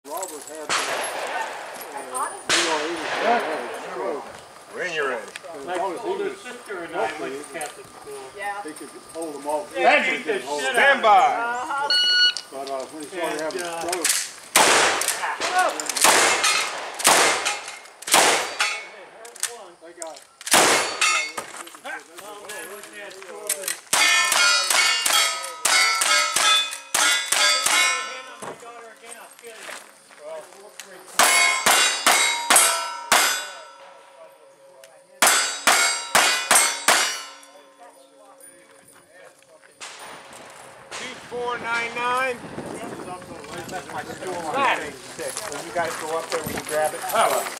Stage 3 Lead Pusher LCP 5 15 21
Lake County Pistoleros May Monthly Match at the Eustis Gun Club in Tavares, FL.

Top 5 Overall:
1) The Outlaw Travis James - WRA – 141.55
2) Lead Pusher – 49er – 148.17
3) Kid Romeo – ES – 163.50
4) Blazing Saddles – GF – 174.45
5) Realtree – D – 176.18

Top 3 Ladies:
1) Lake Picket Lady
2) Chickie Winsome
3) Madame Moog

Clean Shooters:
Marshall Lead Ready, Larry Earp, Poinciana Pete, Bisley, and Lead Pusher

Dagnabbit:
Pawnee James, Pink, Lake Picket Lady, Dead Aim Dave, Realtree, and Kid Romeo